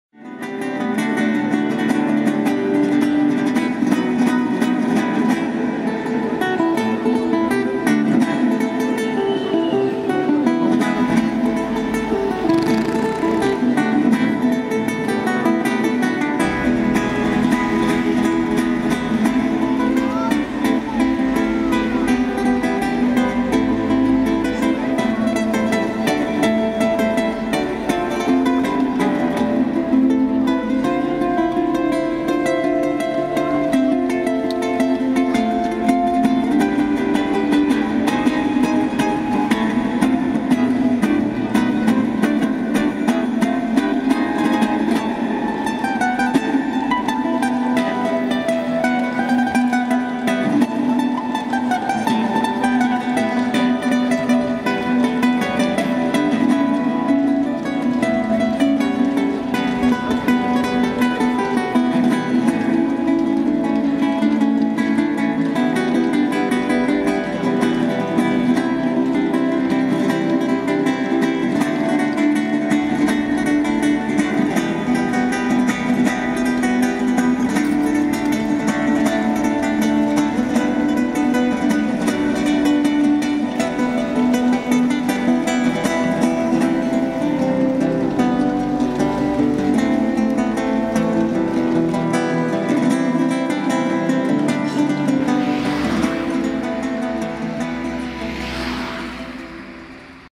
Kiełbaski z niebieskiej Nyski w Krakowie 2017
Co zjeść w Krakowie nocą? Oczywiście kiełbaski z niebieskiej Nyski! Fenomen tego smacznego biznesu tkwi w prostocie, gorąca kiełbaska ze świeżą bułeczką i musztarda oraz oranżada o smaku PRL za dychę! Ponadto w godzinach od 20 do 3 rano trudno w Krakowie zjeść coś równie dobrego na gorąco. Masz gwarancję, że w każdą noc oprócz niedziel i świąt zjesz wyśmienitą gorącą  kiełbaskę z rożna. Gorąco polecamy!
Hrubieszów.TV